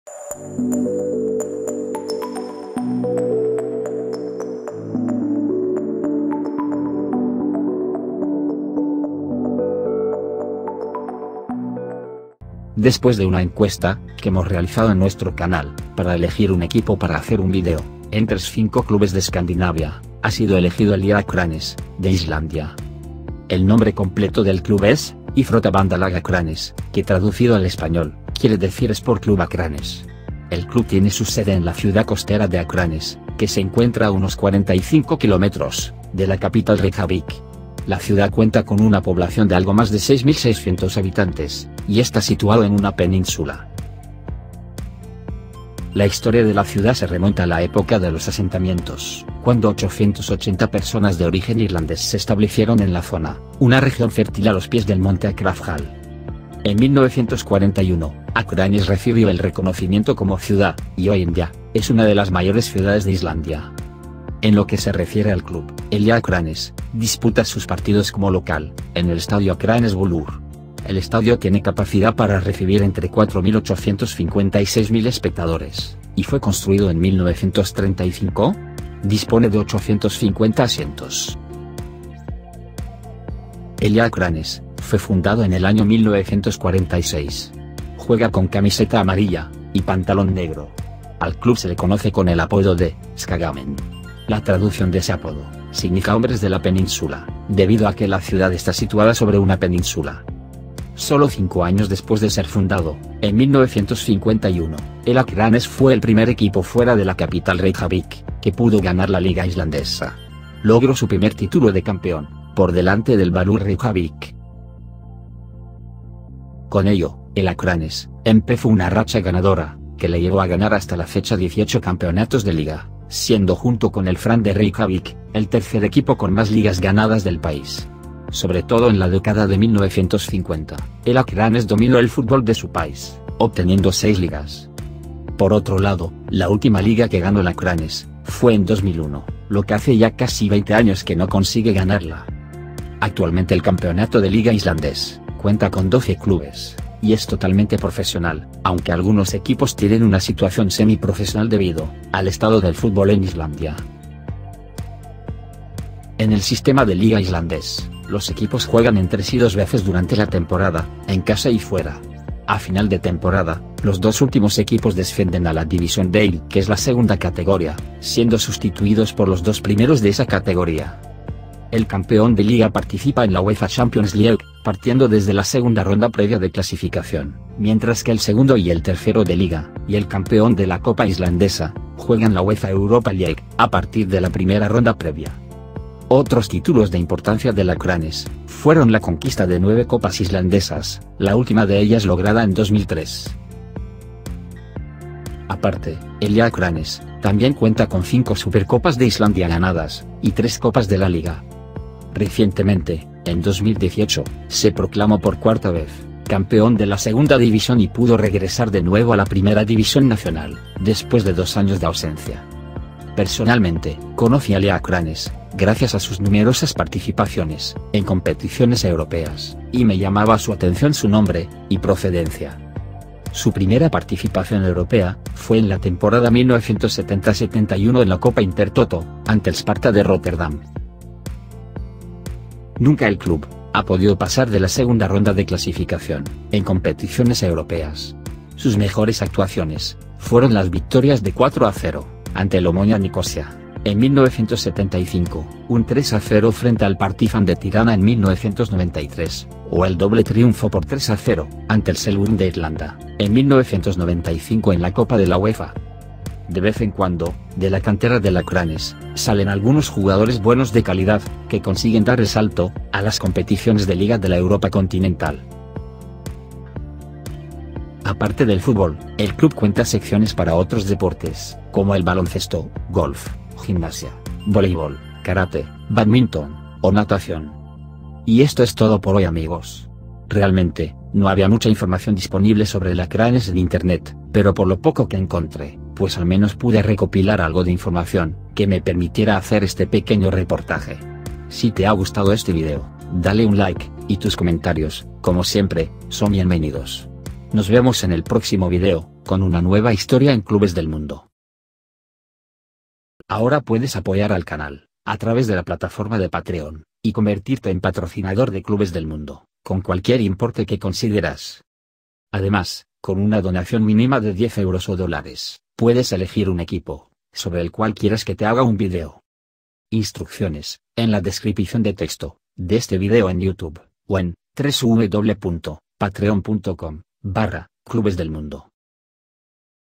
IA AKRANES - Los "Skagamenn" - Uno de los grandes Clubes de Islandia
Conoce en este video, la historia del IA Akranes, uno de los grandes equipos de Islandia. En 1951, fue el primer Club fuera de la capital de Reykjavik, que pudo ganar la Liga de Islandia. Con el tiempo ha llegado a ganar un total de 18 Ligas, lo que le convierte junto con el Fram Reykjavik, en el tercer equipo con más Ligas de Islandia.
Al equipo se le conoce con el sobrenombre de "Skagamenn", lo que traducido quiere decir algo asi como, los hombres de la peninsula, haciendo referencia a que la ciudad de Akranes esta situada en una peninsula, a unos 45 km de Reykjavik.